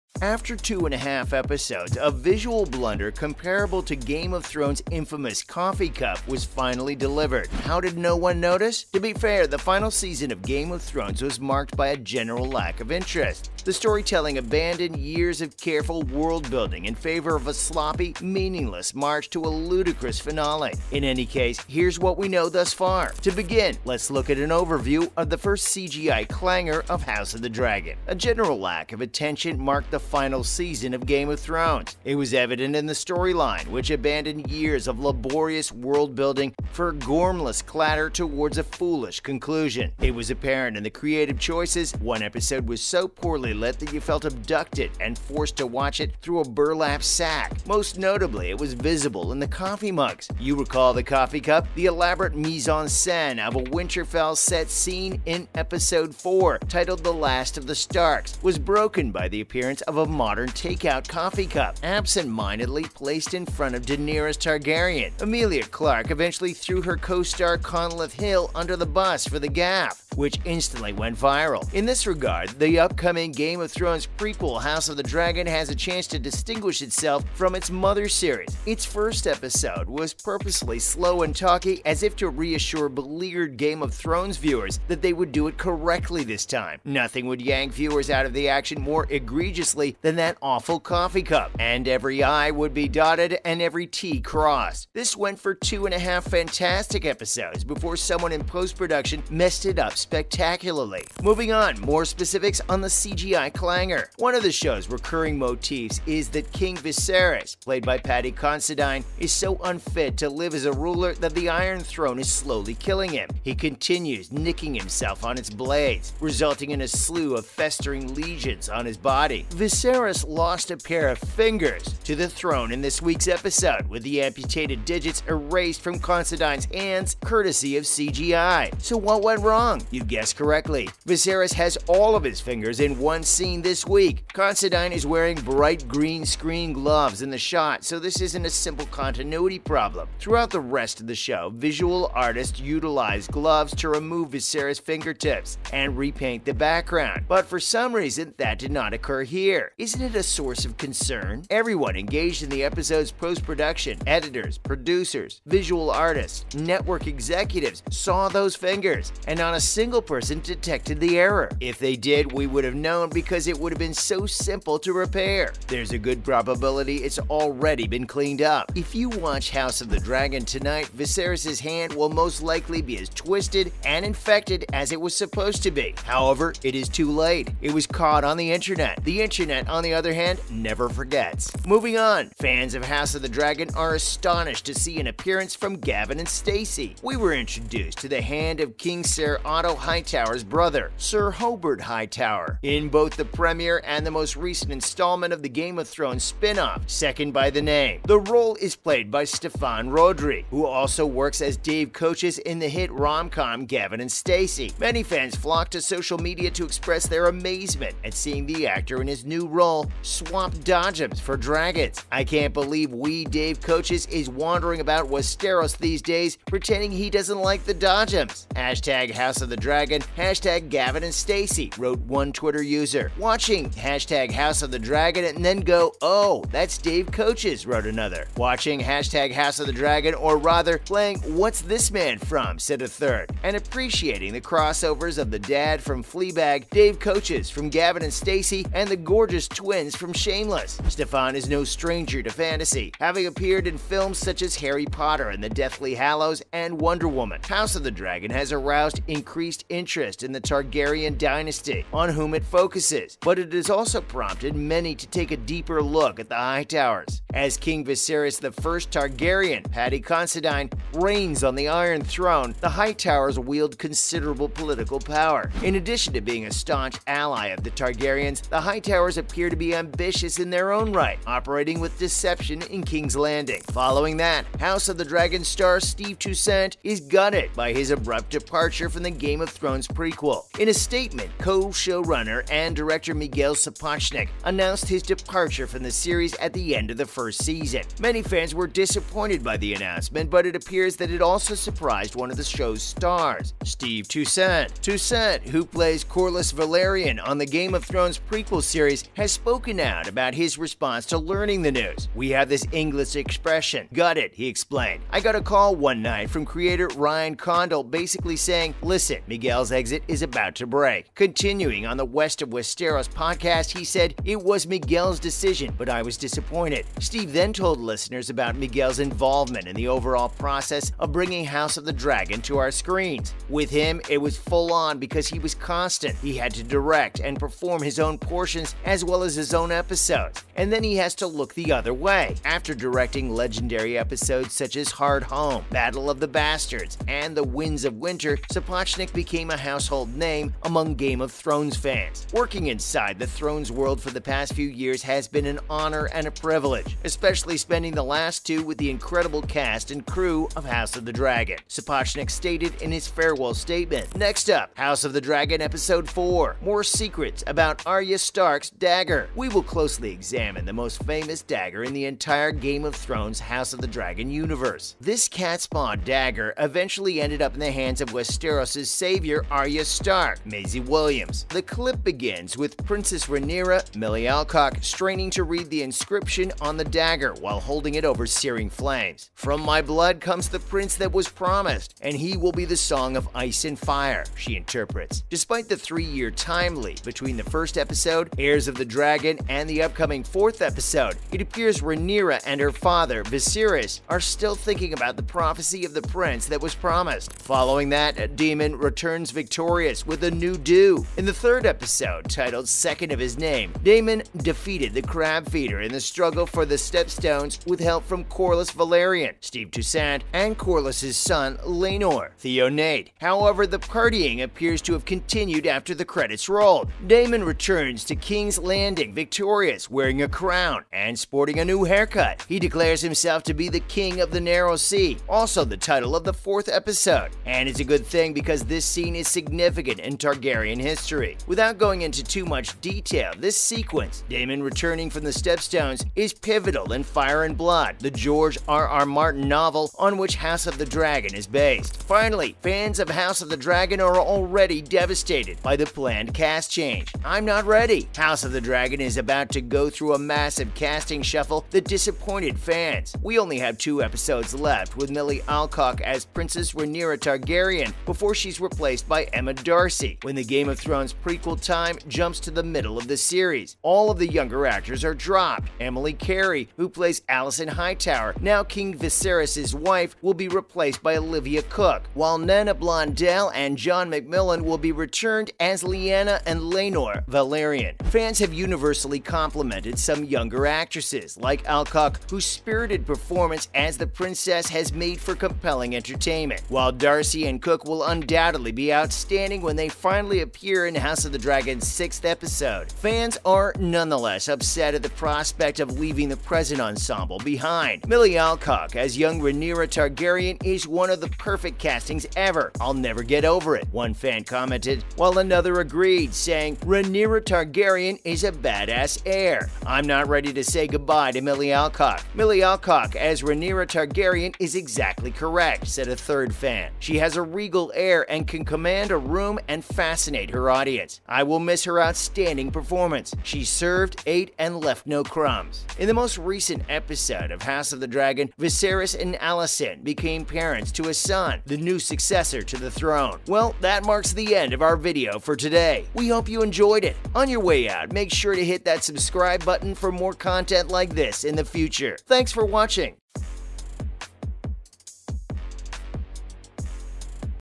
House Of The Dragon CGI Fail Has Been EXPOSED..
Welcome back to Show Focus. The new Game of Thrones prequel House of the Dragon has an opportunity to set itself apart from its mother series. Its first episode was deliberately slow and talky, as if to reassure wounded Game of Thrones fans that this time they were going to do it right. Every i would be dotted, every t crossed, and nothing would take viewers out of the moment as egregiously as that darn coffee cup.

In this week’s episode, it was revealed that Viserys had lost a couple of fingers to the throne, with the amputated digits being airbrushed from Considine’s hands thanks to CGI.  In the rest of the show, visual artists use the gloves to remove Viserys’s fingers and paint the background back in. But here, for whatever reason, that didn’t happen. Meanwhile, Traditional TV viewership of “House of the Dragon” dropped for the first time over the Labor Day holiday weekend. Episode 3, titled “Second of His Name,” was watched by 2.5 million people across its four cable airings on HBO on Sunday, per Nielsen — one million fewer viewers than Episode 2.